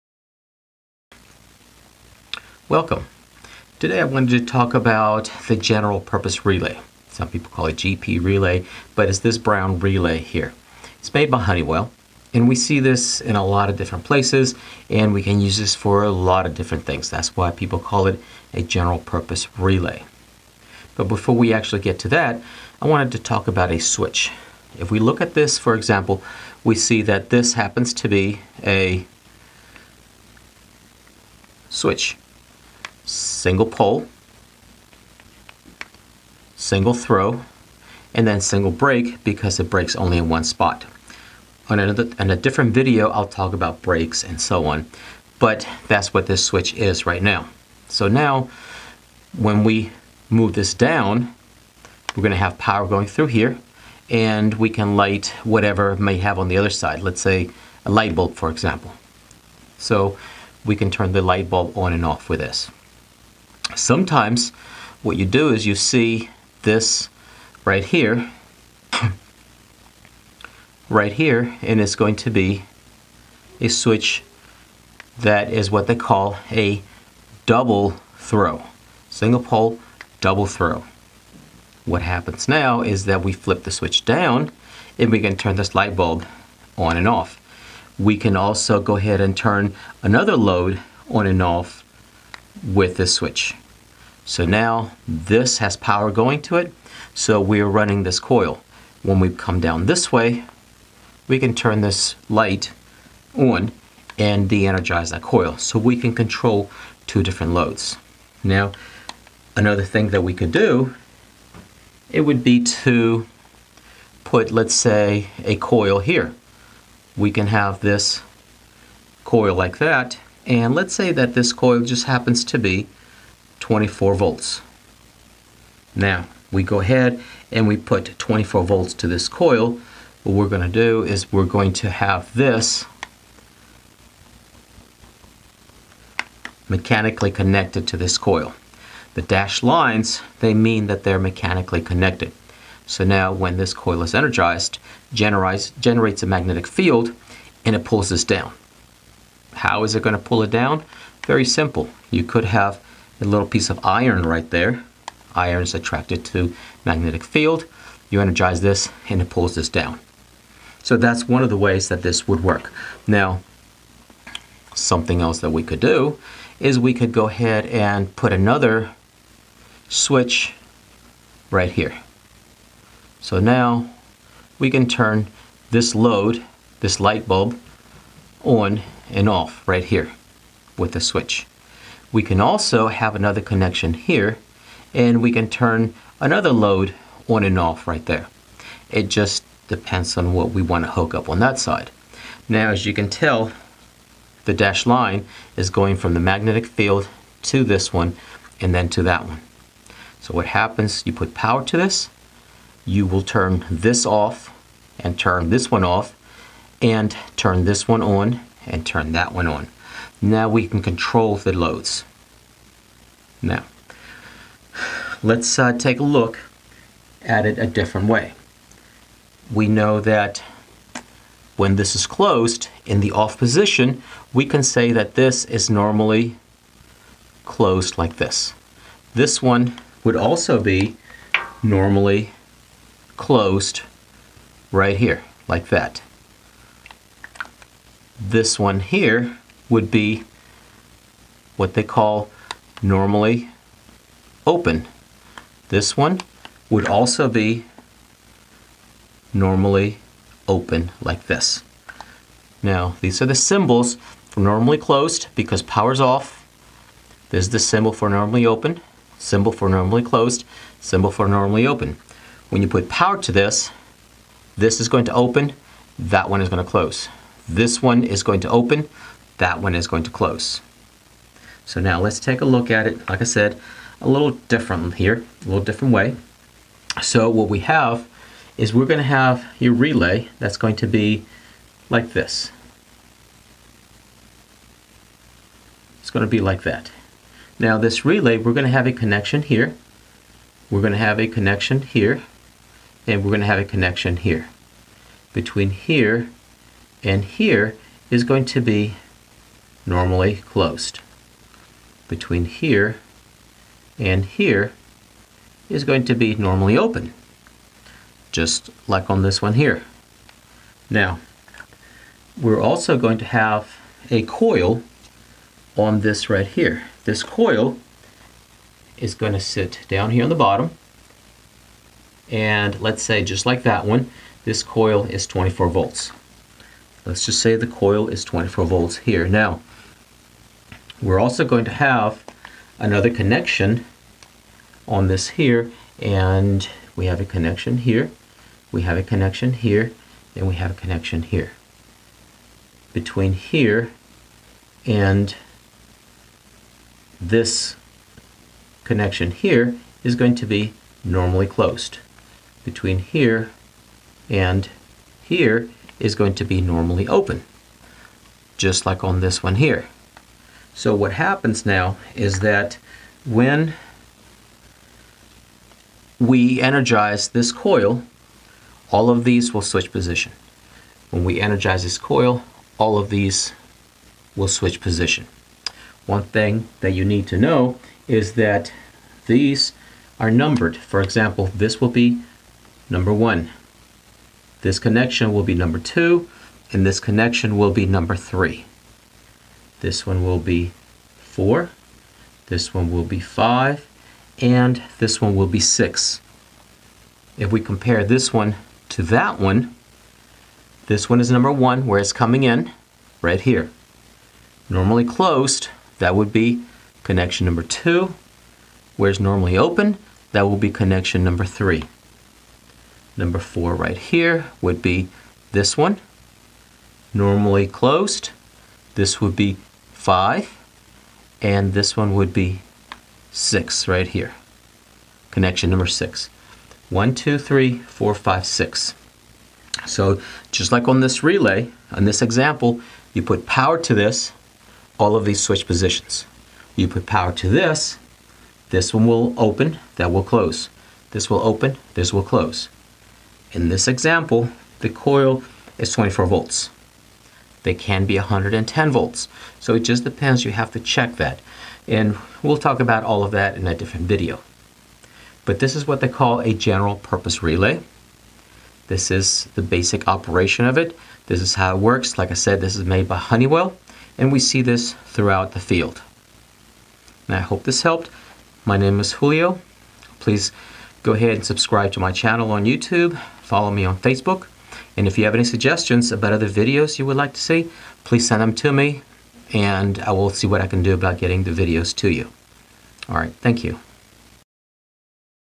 General Purpose Relay Part 1 - HVAC Online Training and Courses
In this video, I explain the connections and uses of a General purpose relay. I explain how it should be hooked up, and ways to use it.